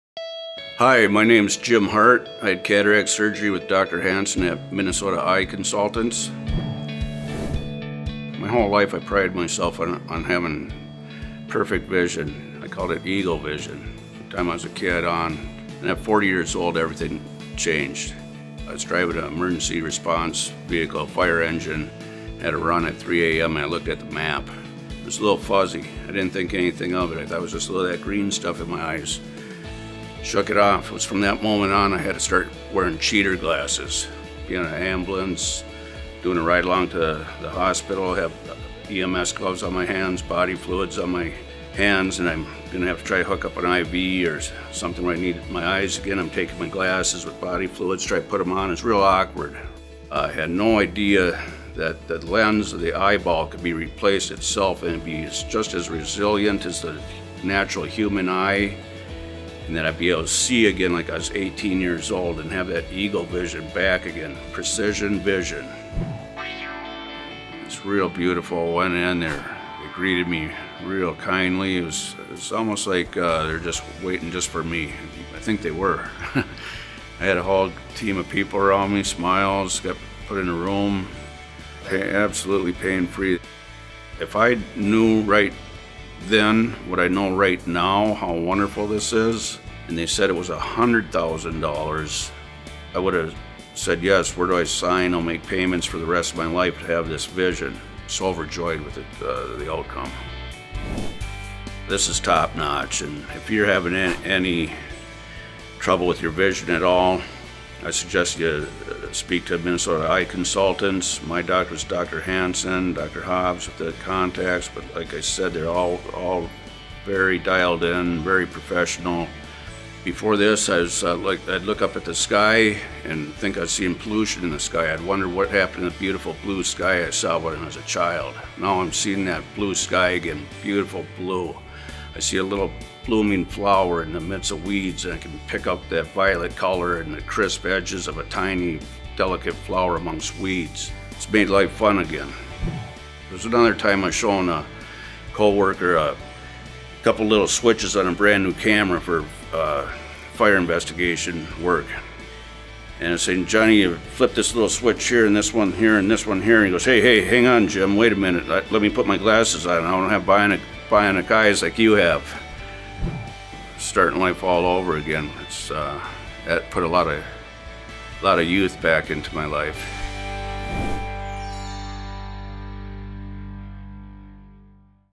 Jim | Cataract & Multifocal IOL Patient
Jim underwent Cataract Surgery with Dr. Mark Hansen and elected the upgraded Multifocal lens. Listen as he talks about some of the environmental challenges he faced as an emergency responder before the surgery, and how the procedure changed everything.